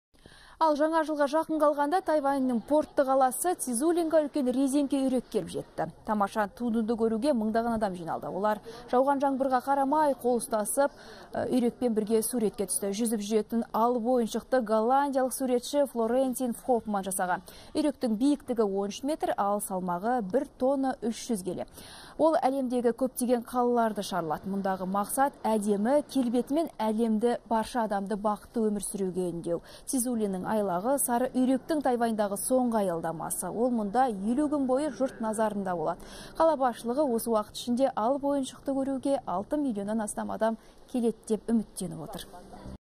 Алып үйрек сапарын аяқтады
Жаңа жылға жақын қалғанда Тайваньның портты қаласы -- Цзилунға үлкен резеңке үйрек келіп жетті.